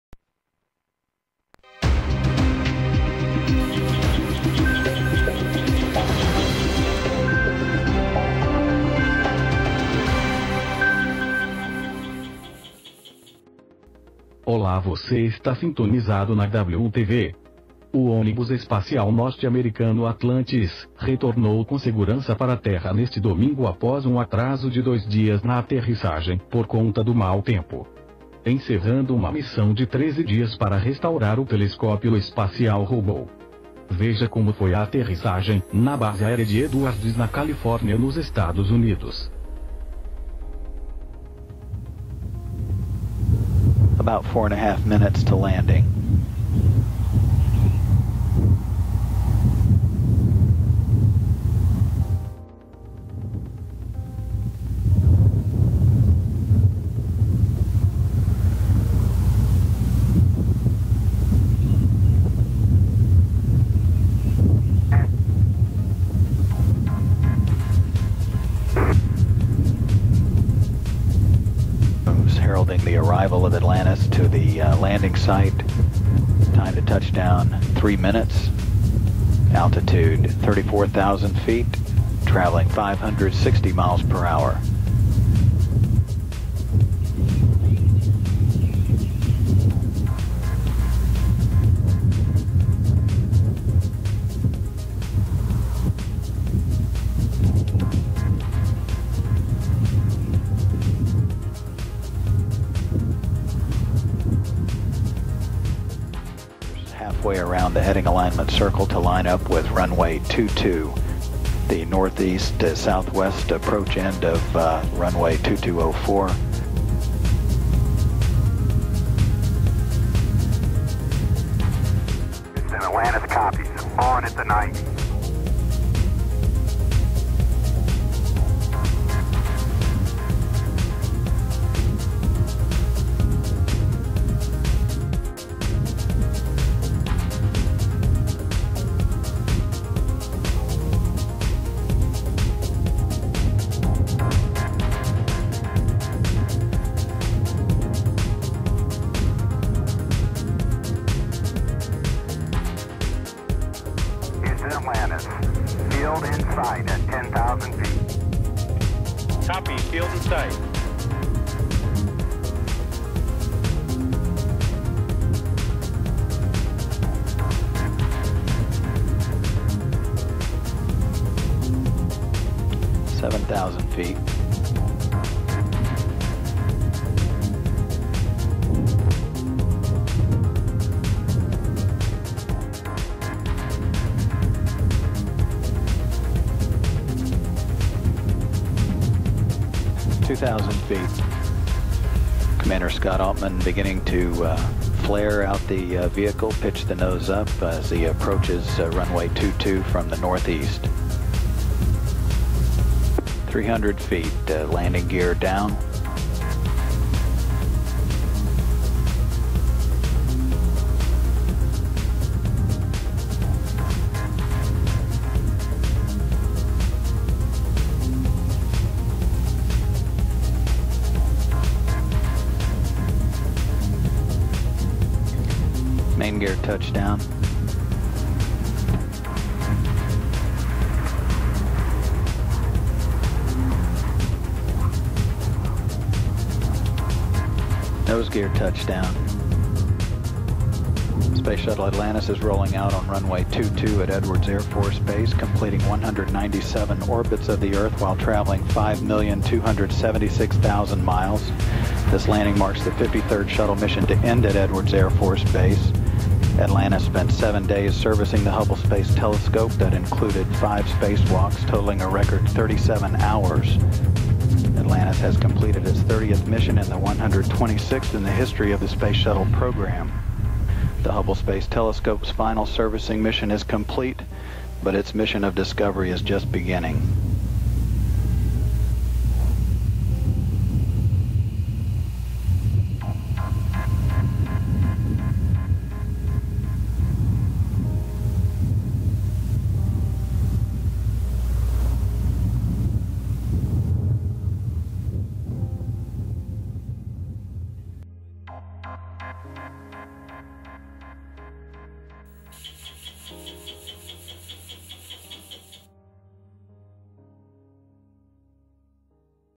Ônibus espacil Atlantis volta a Terra, após missão Hublle - Veja o pouso na base Edwards.
O õnibus espacial Atlantis volta a Terra, após missão Hublle - Veja o pouso na base Edwards.